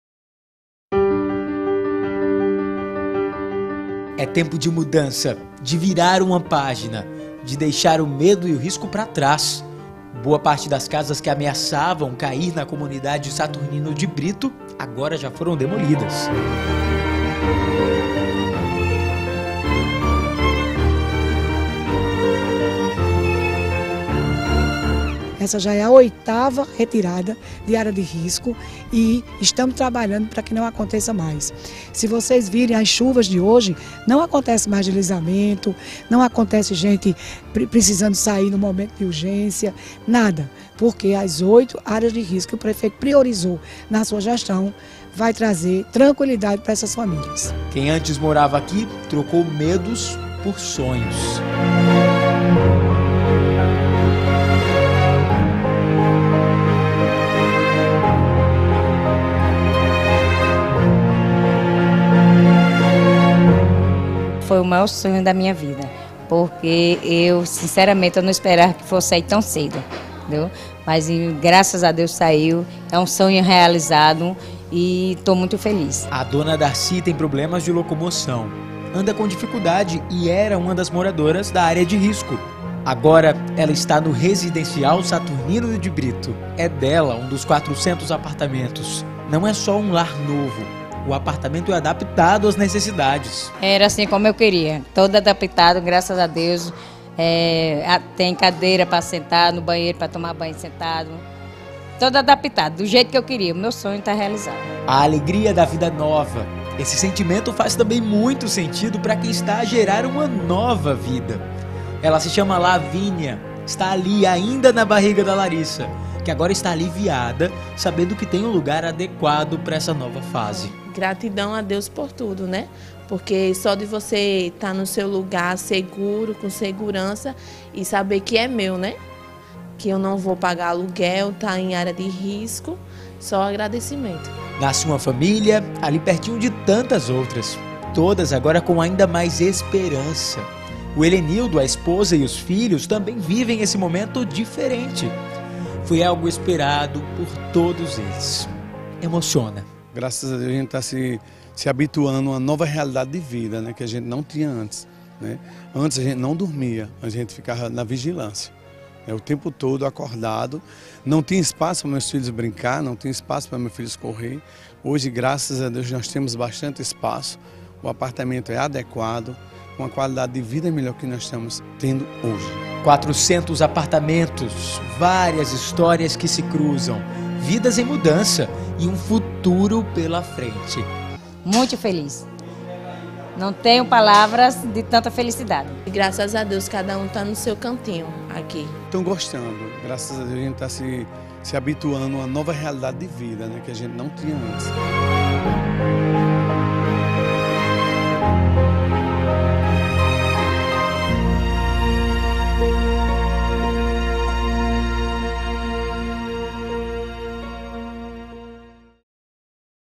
Residencial Saturnino de Brito - Um novo começo
Estar rodeado de tristeza e medo a cada período chuvoso são lembranças que os moradores da Comunidade Saturnino de Brito deixaram definitivamente para trás.

O novo Residencial Saturnino de Brito - entregue recentemente pela Prefeitura de João Pessoa - mudou a vida de 1600 pessoas, que agora não têm apenas a casa própria, mas dignidade.

E as conquistas não ficaram só nas paredes do habitacional. O entorno recebeu toda infraestrutura e melhorias em saneamento básico, além de pavimentação, drenagem e área de lazer.

Vamos conhecer essas histórias de superação, recomeço e felicidade pelas lentes da sua TV Cidade João Pessoa. 😍